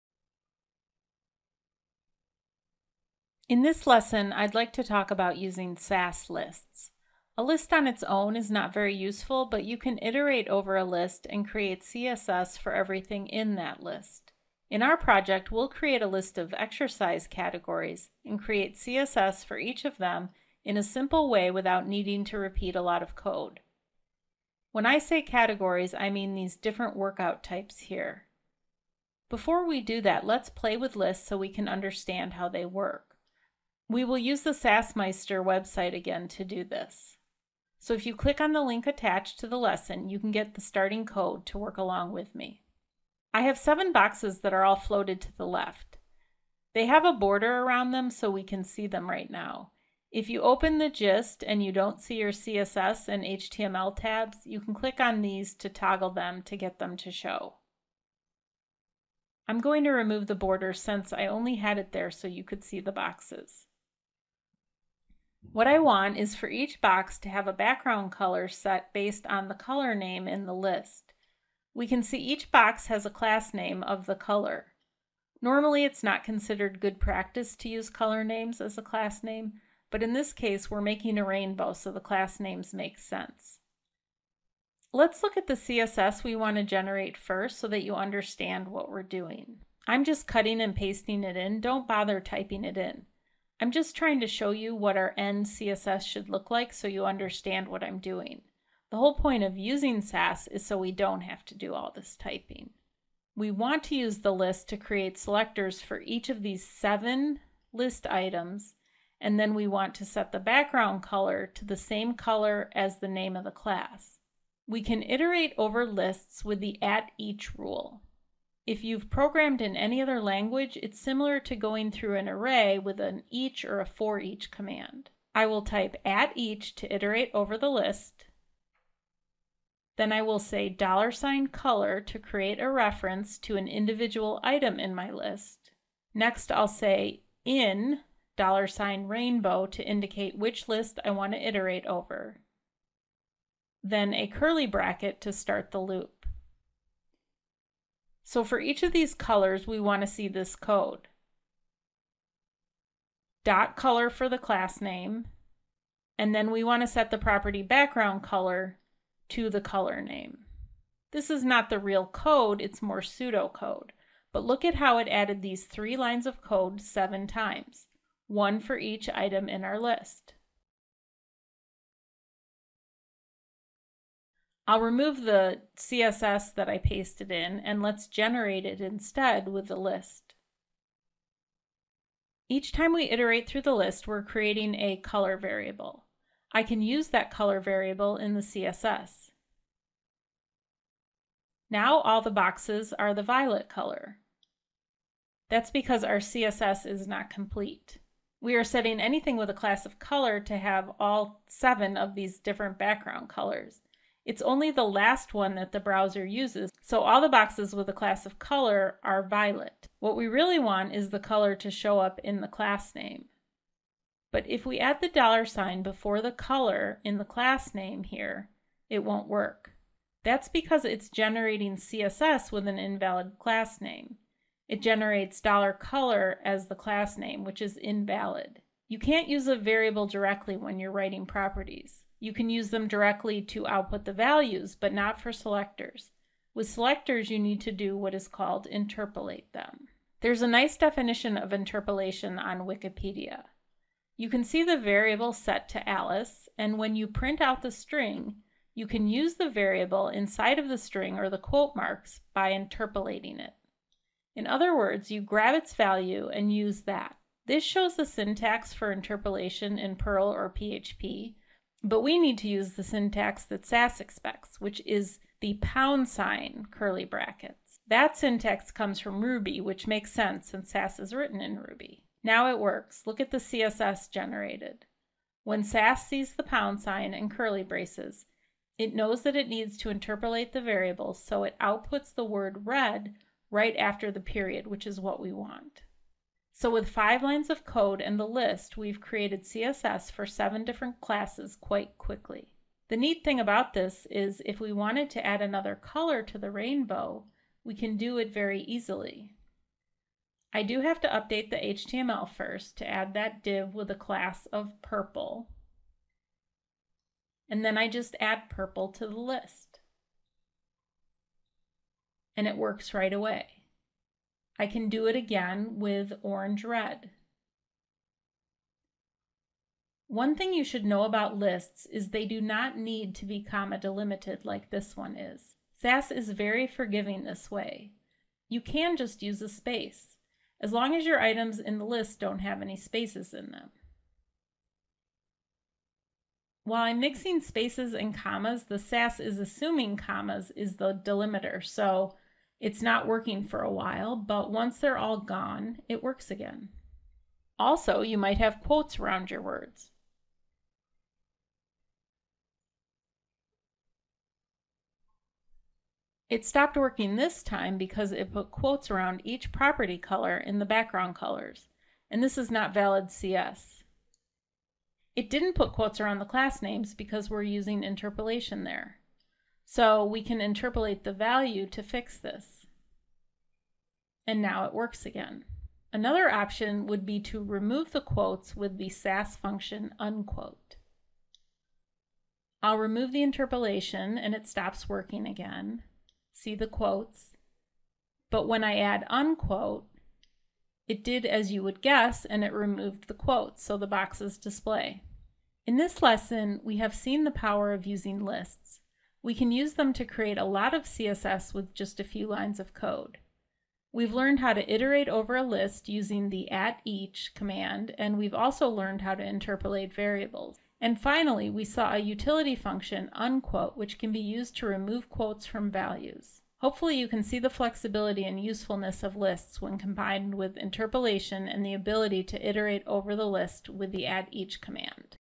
Sass Lists
Sass Lists can be used to generate lots of CSS with very little code.

This video is a sample from my upcoming Sass course The Next Step with Sass and Compass. It will available at Learnable.com shortly and more places in the future.

This video also shows the usefulness of the SassMeister.com website which is a Sass code playpen.

You can follow along with the video by using this